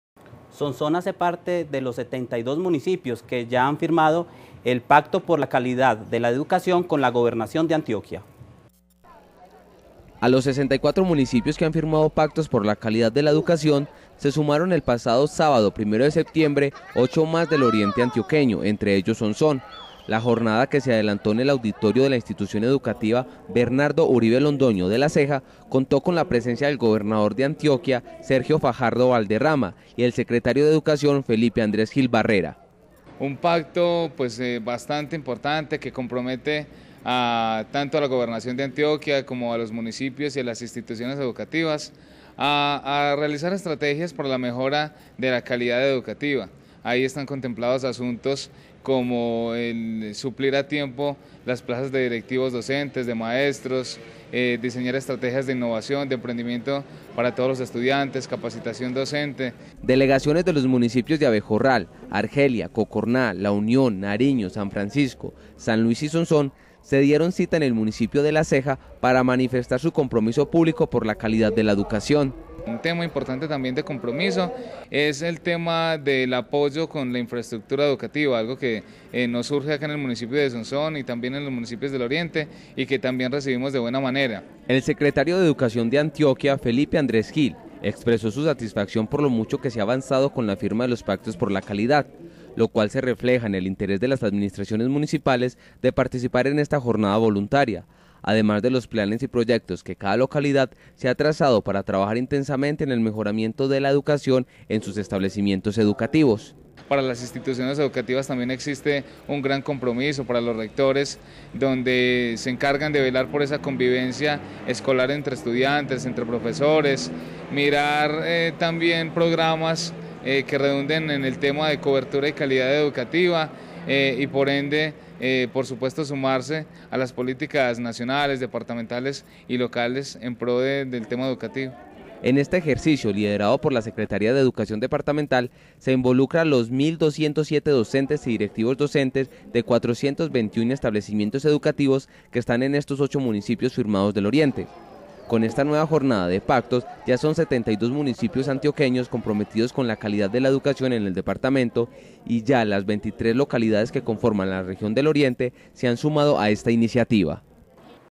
Sonsón hace parte de los 72 municipios que firmaron Pacto por la Calidad de la Educación
A los 64 municipios que han firmado Pactos por la Calidad de la Educación, se sumaron el pasado sábado primero de septiembre ocho más del Oriente antioqueño entre ellos Sonsón. La jornada que se adelantó en el auditorio de la Institución Educativa Bernardo Uribe Londoño de La Ceja, contó con la presencia del Gobernador de Antioquia, Sergio Fajardo Valderrama y el Secretario de Educación, Felipe Andrés Gil Barrera.